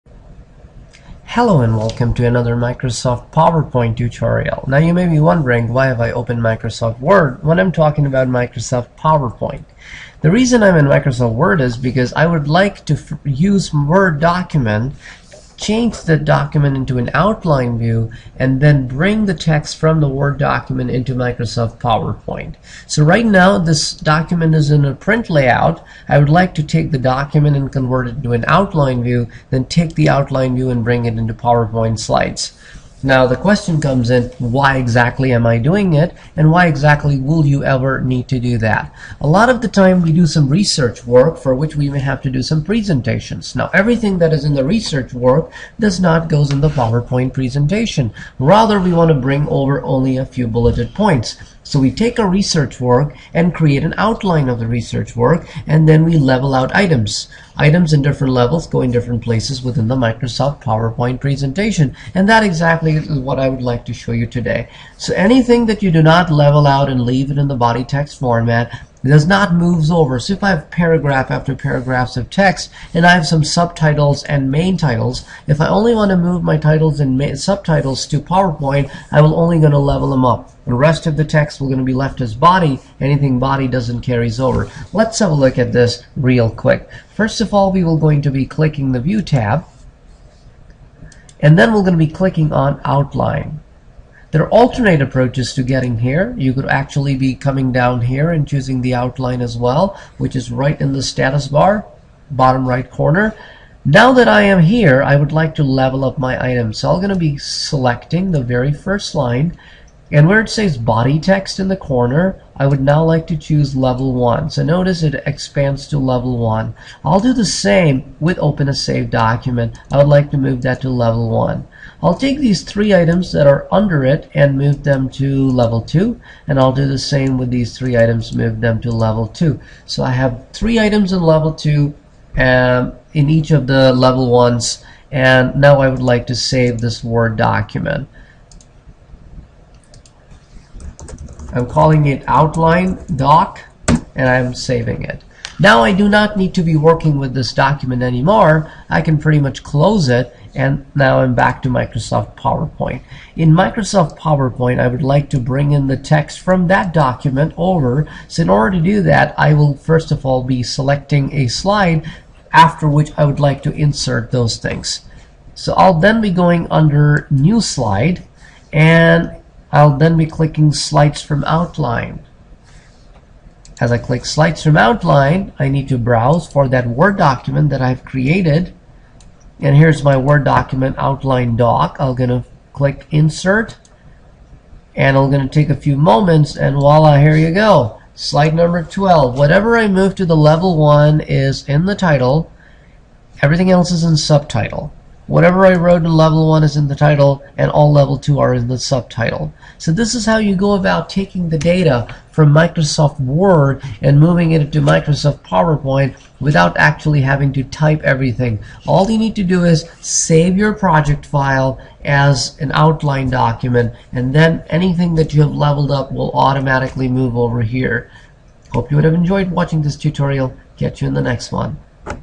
MS PowerPoint Tutorial - Create Slides from MS Word Outline
This tutorial teaches you how to create Slides from MS Word Outline